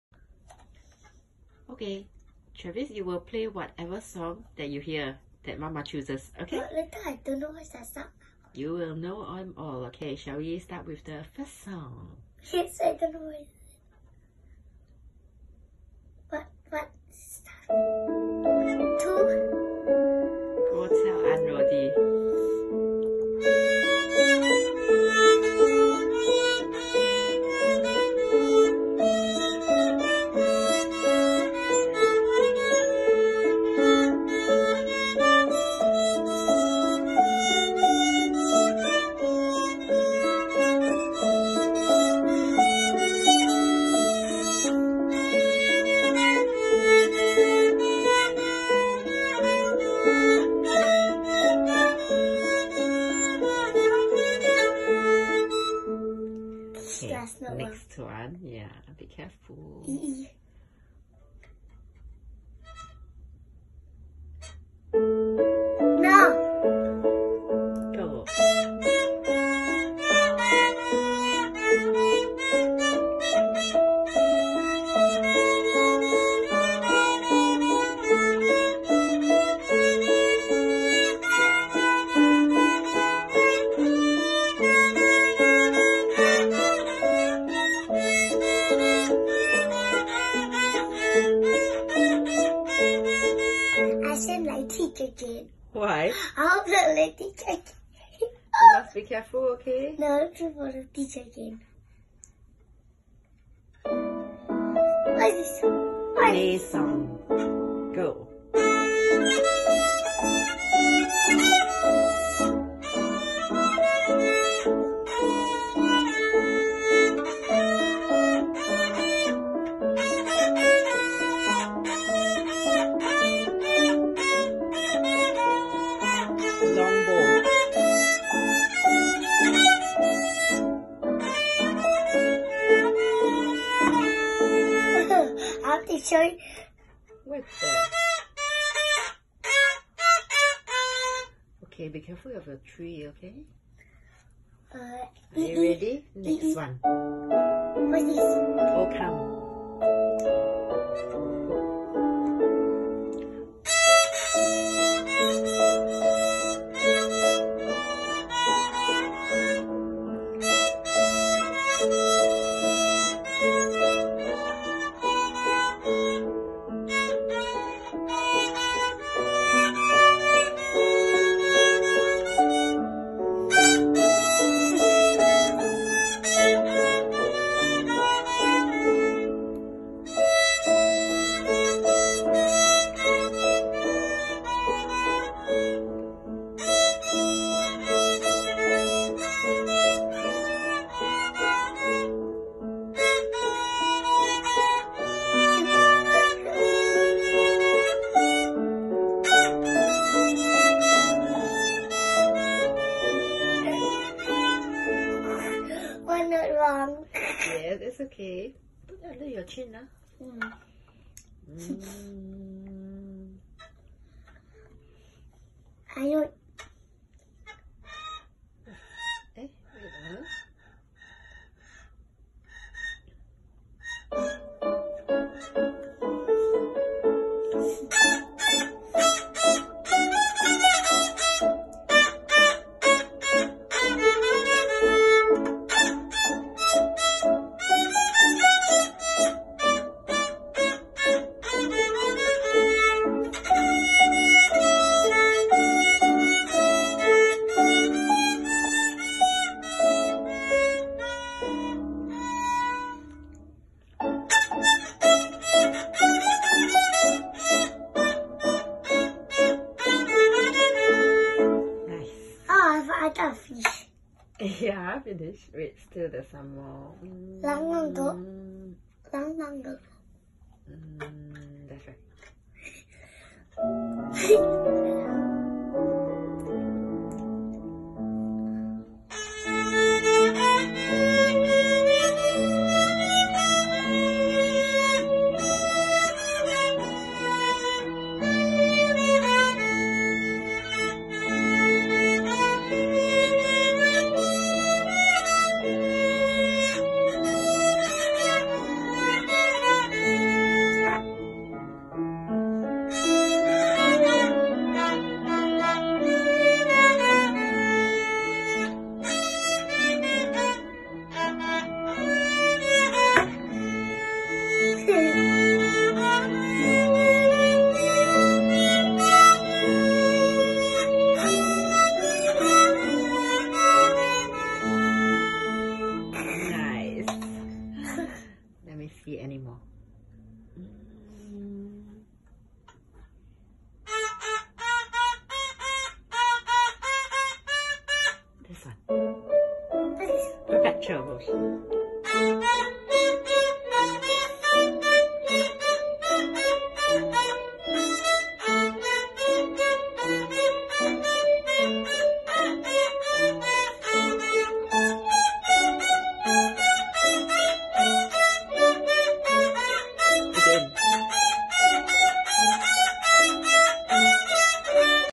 4 years old Travis Wong playing violin Random Songs Challenge - 6 Aug 2019
4 years old Travis Wong playing violin Random Songs Challenge 6 Aug 2019